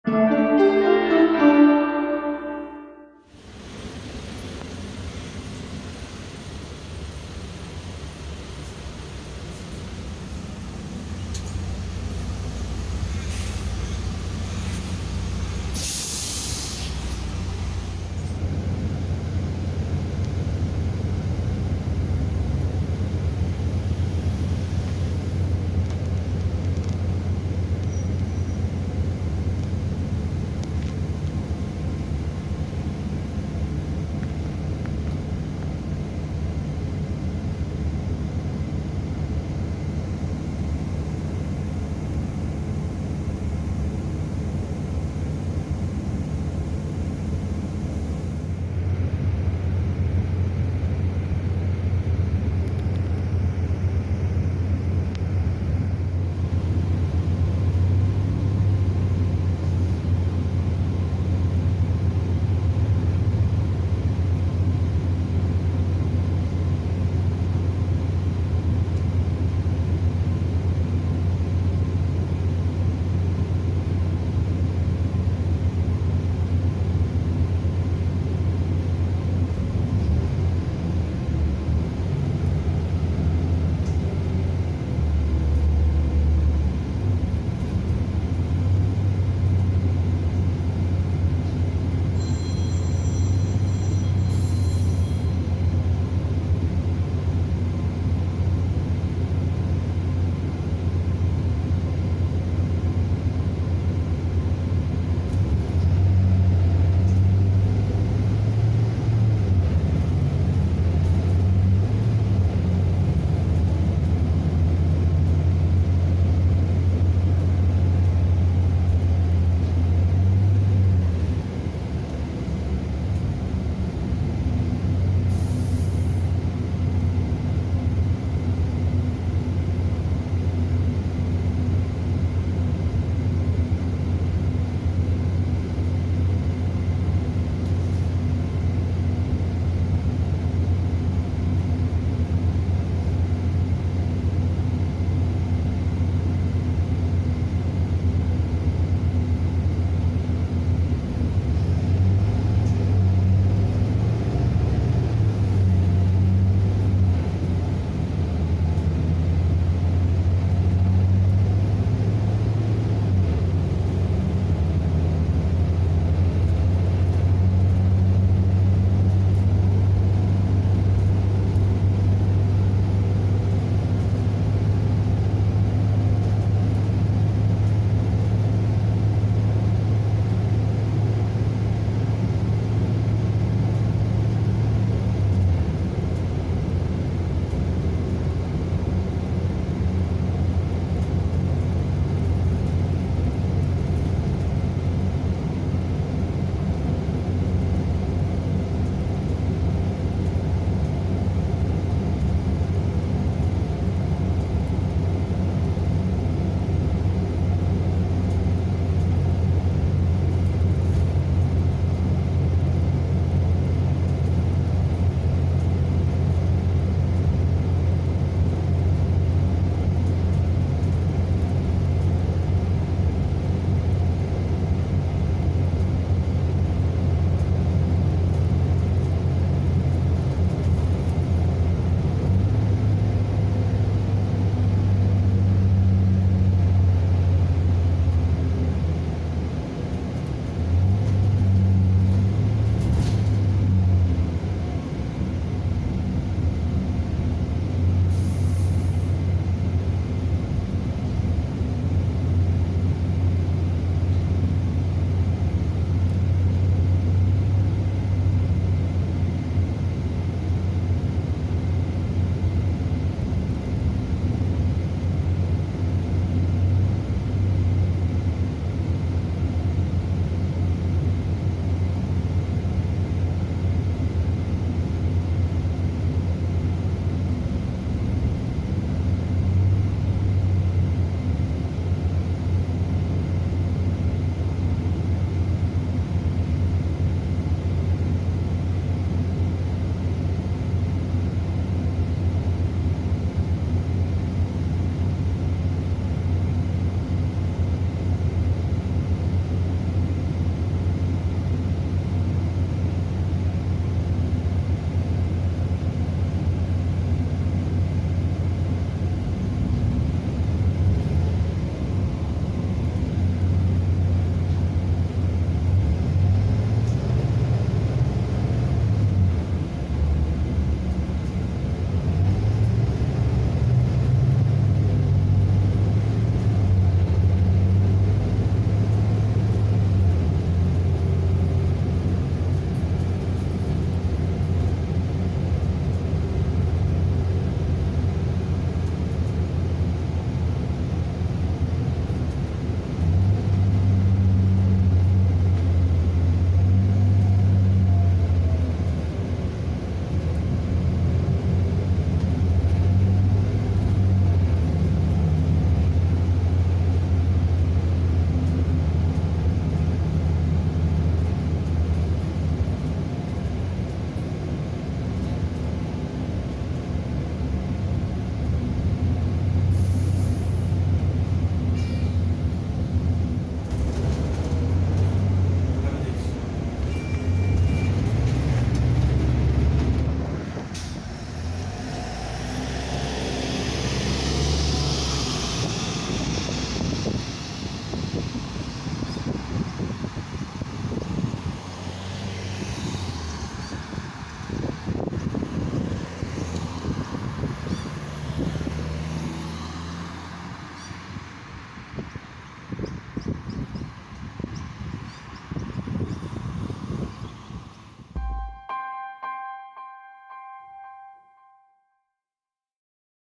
A trip on Kastoria Bus Lines Volvo B8RLE (Bustech VST) #12
I quite like the blue interior of these Kastoria buses! Makes it very unique to the Melbourne bus fleets.
Kastoria is the only bus company in Melbourne that has VST buses in service. This particular bus that I'm on was registered in January 2021, which is rather new.
All of the Kastoria VST's are fitted with either a Volvo B7RLE or a B8RLE chassis.